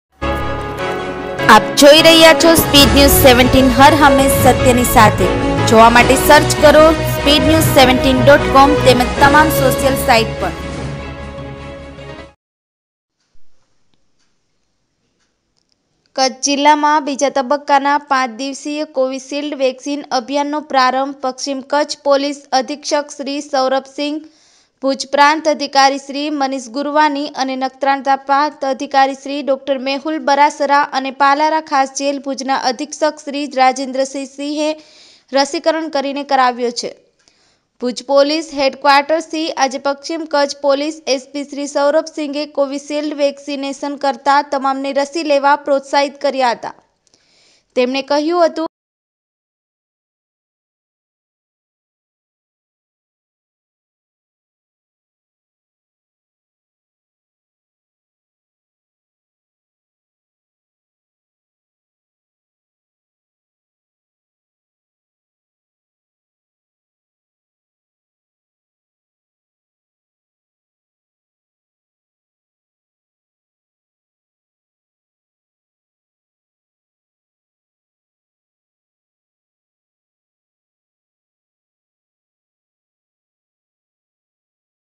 KUTCH જિલ્લામાં બીજા તબક્કાના પાંચ દિવસીય કોવિશિલ્ડ વેકસીન અભિયાનનો પ્રારંભ પશ્ચિમ કચ્છ પોલીસ
નોવેલ કોરોના વાયરસ કોવીડ-૧૯
કોવિશિલ્ડ વેકસીનના બીજા તબક્કાના પાંચ દિવસીય રસીકરણનો કાર્યક્રમ પ્રારંભ
૦ ૦ ૦ ૦
પશ્ચિમ કચ્છ એસ.પી.શ્રી સૌરભસિંઘ, પ્રાંત અધિકારીશ્રી મનીષ ગુરવાની અને ડો.મેહુલ બરાસરાએ કરાવ્યું રસીકરણ
૦ ૦ ૦ ૦
પાલારા  ખાસજેલ અધિક્ષકશ્રી રાજેન્દ્રસિંહની પણ રસીકરણ માટે અપીલ
૦ ૦ ૦ ૦
આ રસી સંપૂર્ણ સુરક્ષિત અને સલામત છે – પશ્ચિમ કચ્છ એસ.પી.શ્રી સૌરભસિંઘ
૦૦૦૦૦૦
ભુજ, રવિવારઃ
કચ્છ જિલ્લામાં બીજા તબક્કાના પાંચ દિવસીય કોવિશિલ્ડ વેકસીન અભિયાનનો પ્રારંભ પશ્ચિમ કચ્છ પોલીસ અધિક્ષકશ્રી સૌરભસિંઘ, ભુજ પ્રાંત અધિકારીશ્રી મનીષ ગુરવાની અને નખત્રાણા પ્રાંત અધિકારીશ્રી ડો.મેહુલ બરાસરા અને પાલારા ખાસજેલ ભુજના અધિક્ષકશ્રી રાજેન્દ્રસિંહે રસીકરણ કરીને કરાવ્યો છે.
ભુજ પોલીસ હેડકવાર્ટસથી આજે પશ્ચિમ કચ્છ પોલીસ એસ.પી.શ્રી સૌરભસિંઘે કોવિશિલ્ડ વેકસીનેશન કરતાં તમામને રસી લેવા પ્રોત્સાહિત કર્યા હતા. તેમણે કહયું હતું કે, ‘‘વૈશ્વિક મહામારી કોરોના માટેની વેકસીન સુરક્ષિત અને સલામત છે. વેકસીન બાદ સૌ નોર્મલ અનુભવે છે. તો સૌ આ રસી લઇએ અને કોરોના મહામારીનો એક સાથે મળી સામનો કરીએ.’’
પાલારા ખાસજેલ ભુજના અધિક્ષકશ્રી રાજેન્દ્રસિંહે પણ રસી લઇ સૌને આ અભિયાનમાં જોડાવા અપીલ કરી હતી.
ભુજ પ્રાંત અધિકારીશ્રી મનીષ ગુરવાનીએ પણ આજે કોવિશિલ્ડ વેકસીનેશન લઇ, કોરોના ફ્રન્ટલાઇન વોરિયર્સ, વર્કસને અનુરોધ કર્યો હતો કે, ‘‘સૌ અભિયાનનો ભાગ લઇ સુરક્ષિત બની કોરોના કોવીડ-૧૯ની ગાઇડલાઇનનું પાલન કરીએ.’’
પાના નં.(૨) ઉપર

પાના નં.(૨)
નખત્રાણા પ્રાંત અધિકારી ડો.મેહુલ બરાસરાએ નખત્રાણા ખાતે રસી લેતાં સૌને અનુરોધ કર્યો હતો કે, કોઇપણ ડર વગર નિયત શીડયુલ પ્રમાણે દરેકે આ વેકસીન લેવી જોઇએ.
પશ્ચિમ કચ્છ ભુજ ડીવીઝન ડેપ્યુટી એસ.પી.શ્રી જે.એન.પંચાલે પણ રસીકરણ લેતા સૌને કોવિશિલ્ડ વેકસીન લેવા અનુરોધ કરી કોરોના મહામારીને અટકાવવા આ રસી લેવી જરૂરી છે તેની કોઇ આડઅસર પણ નથી તેમ જણાવ્યું હતું.
ભુજ તાલુકા ઇન્ચાર્જ તાલુકા હેલ્થ ઓફિસર ડો.અમીન અરોરાએ જણાવ્યું હતું કે, ‘‘પાંચ દિવસના આ રસીકરણ કાર્યક્રમ ૧૬ ટીમો દ્વારા દસ સ્થળોએ યોજાશે. જેમાં ફ્રન્ટલાઇન કોરોના વોરિયર્સ આરોગ્ય બાદ હવે પોલીસ, મહેસુલ, તાલુકા પંચાયત, ગ્રામ રક્ષકદળ અને નગરપાલિકાના થઇ અંદાજે કુલ ૬૬૩૫ લોકોને રસી અપાશે.
પોલીસ હેડકવાર્ટસ ભુજ ખાતે યોજાયેલા કોવિશિલ્ડ વેકસીનેશનમાં પોલીસ અધિકારી/કર્મચારીઓએ વેકસીનેશન લીધી હતી જેનું આયોજન એસ.એમ.ચૌહાણ પોલીસ સબ ઈન્સપેકટર દ્વારા કરવામાં આવ્યું હતું.
રસી લેનારા વિવિધ કોરોના વોરિયર્સનો અભિપ્રાય હતો કે આ રસી સંપૂર્ણ સુરક્ષિત અને સલામત છે.
આ તકે રસી લેનારા ફ્રન્ટલાઇન કોરોના વોરિયર્સ સાથે અધિક જિલ્લા આરોગ્ય અધિકારી ડો.પ્રેમકુમાર કન્નર, મુખ્ય જિલ્લા આરોગ્ય અધિકારી ડો.જનક માઢક તેમજ રાષ્ટ્રીય બાલ સ્વાસ્થ્ય કાર્યક્રમના ડોકટરો અને નર્સો તેમજ મીડીયા કર્મીઓ ઉપસ્થિત રહયા હતા.
એચ.પારેખ/સીદીક કેવર

સ્ટોરી:-રમેશ ભાનુશાલી કચ્છ